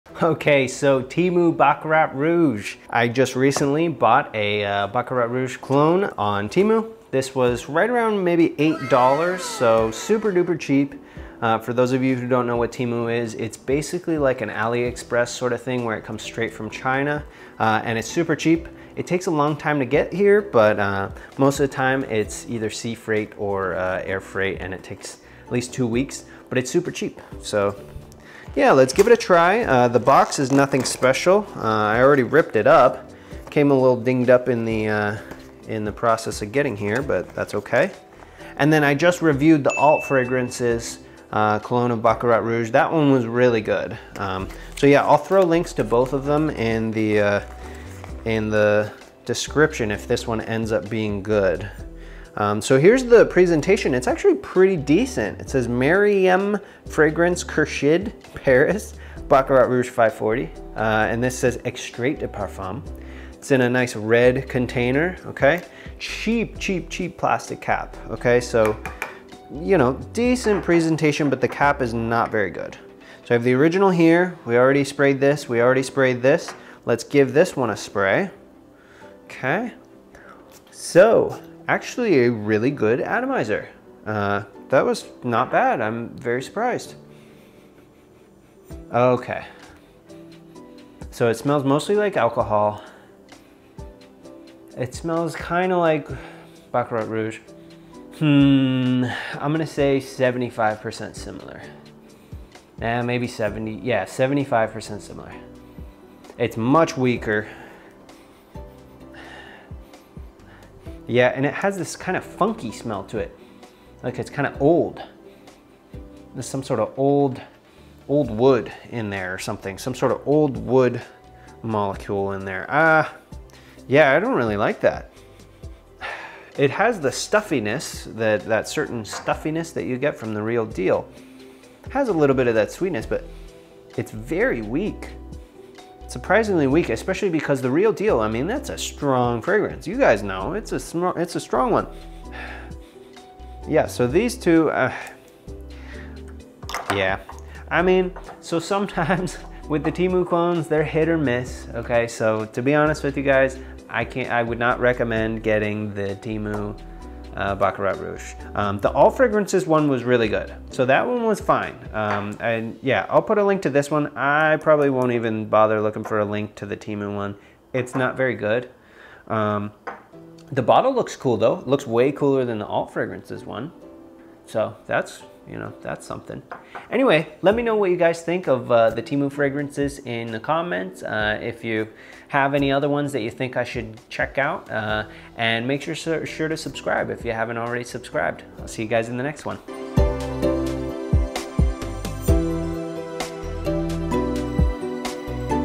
TEMU Baccarat Rouge: Worth Buying??
Hey everyone! This is another installment of the TEMU fragrance clone reviews- I hope you enjoy!

my microphone
my softbox lighting kit
my camera kit
my tripod